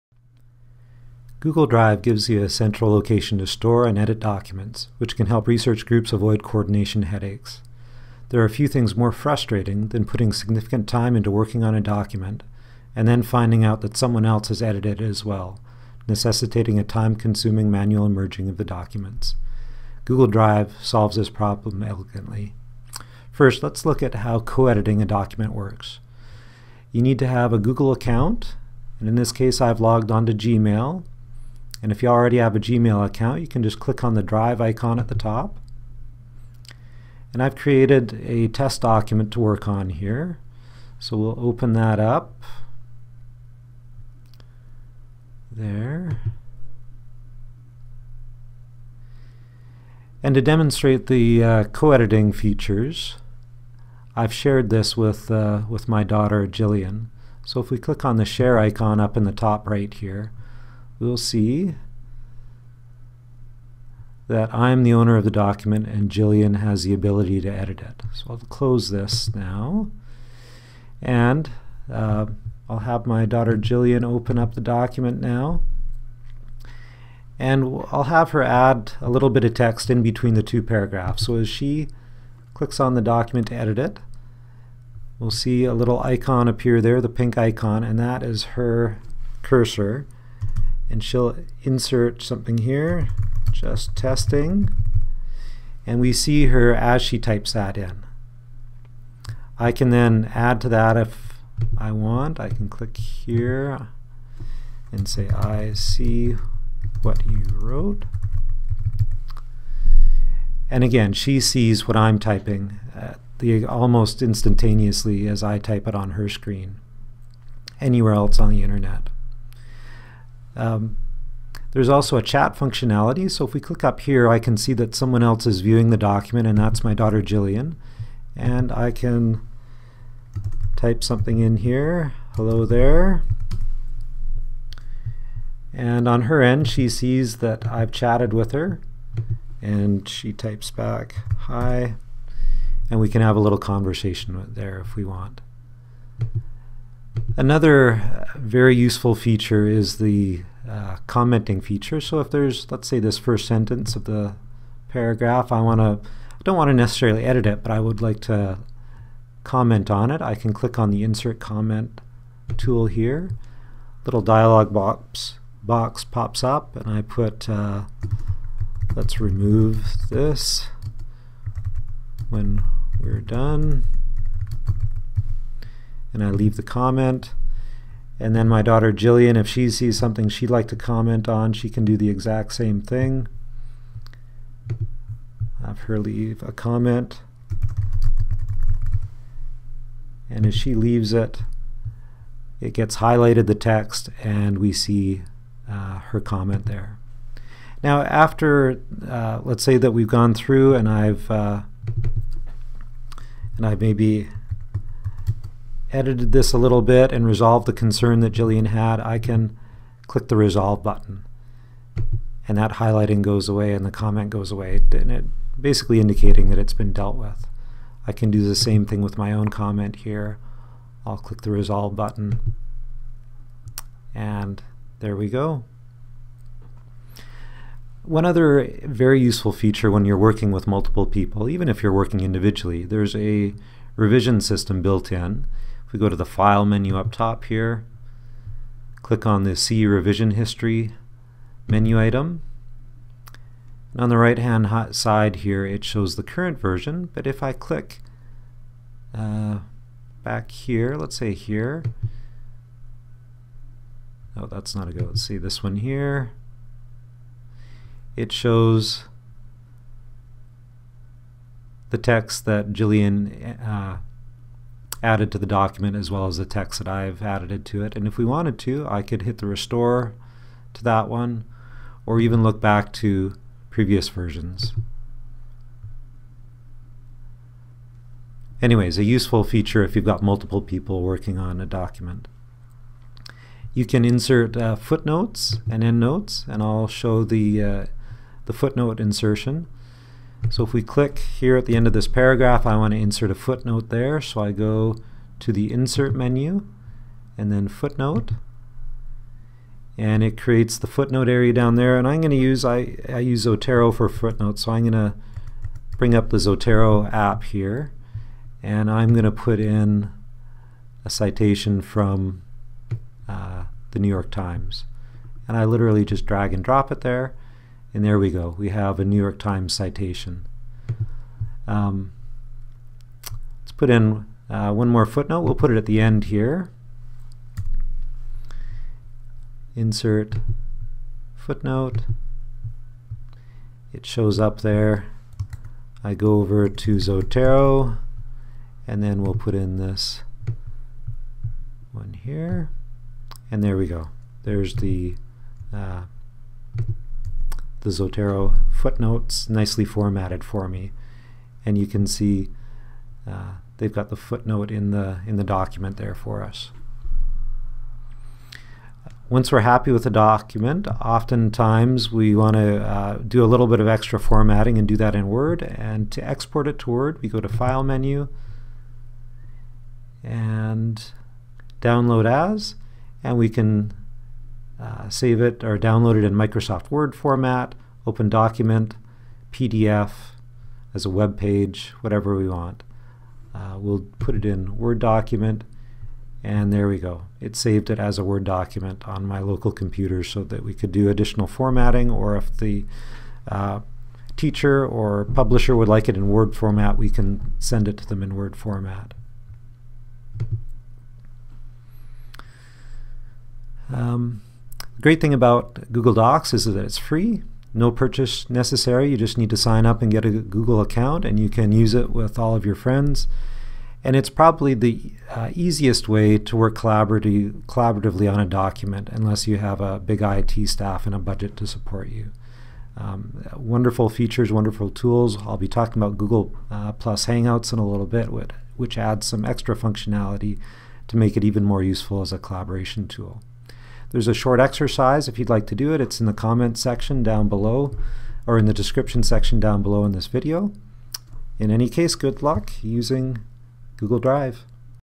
Knowledge Management & Research Tools: Google Drive - Collaborative Document Editing
Google Drive gives you a central location to store and edit documents which can help research groups avoid coordination headaches. There are few things more frustrating than putting significant time into working on a document, and then finding out that someone else has edited it as well, necessitating a time consuming manual merging of the documents.  Google Drive solves this problem elegantly.

Exercise:
- If you haven't already, create a Gmail account for your self (at Gmail.com) and then log on.  Press the "DRIVE" icon on the top menu.
- Paste a couple of paragraphs from a paper you've written into the document.
- Insert two footnotes & drag & drop references from Zotero.
- Share the document with a classmate or friend.
- Edit the document at the same time as them.